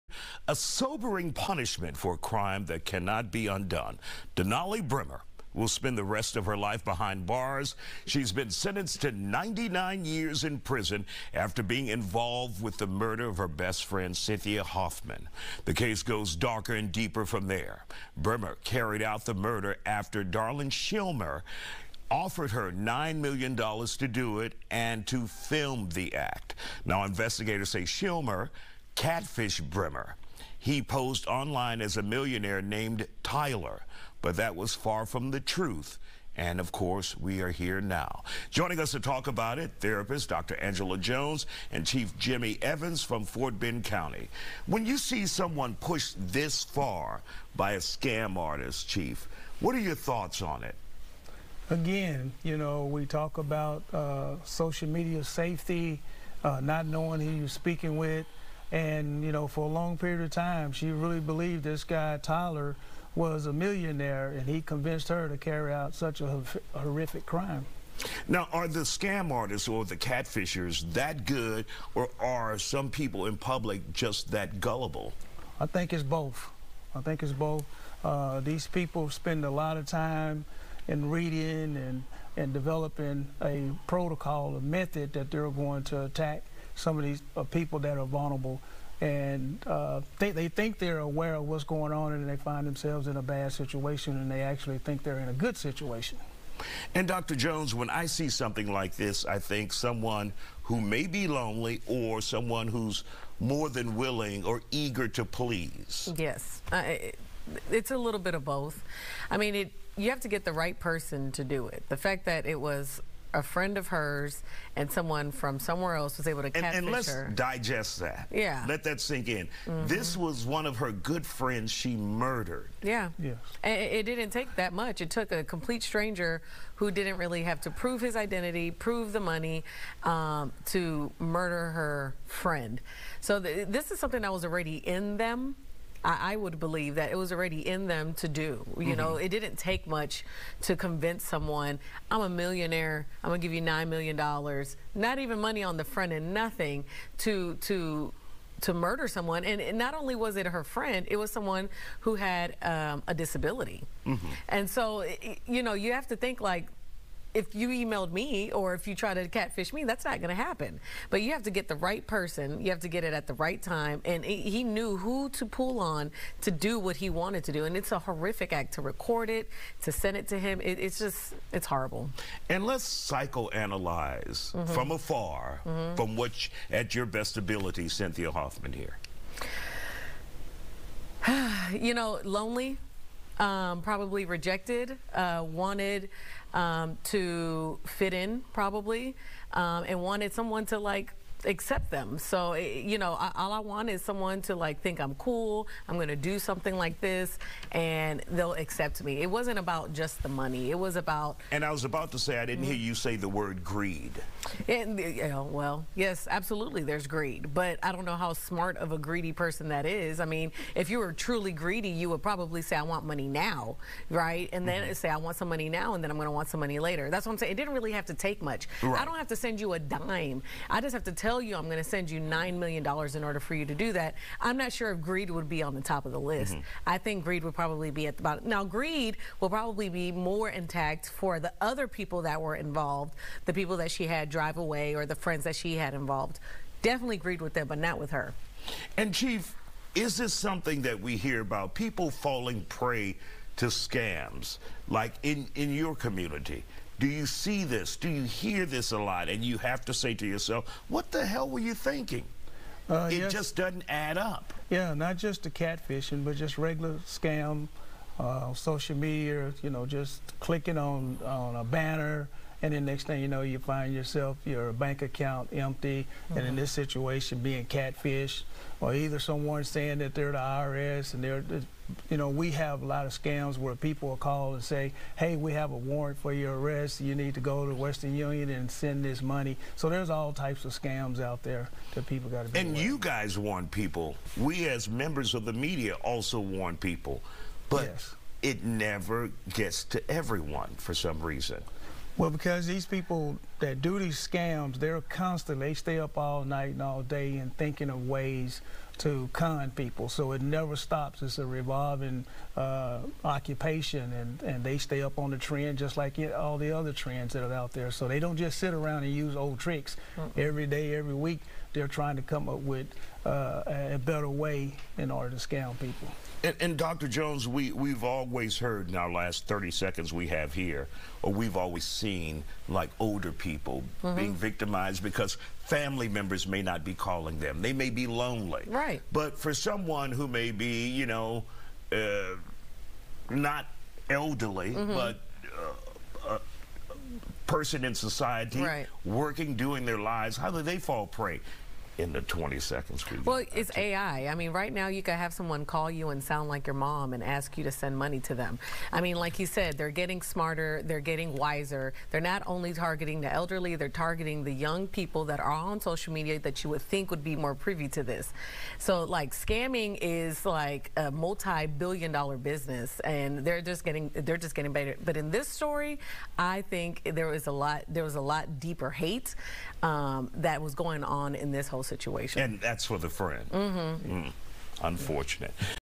Woman sentenced to 99 years in prison for catfish murder plot
A woman is handed down a punishment after helping kill someone for an online boyfriend.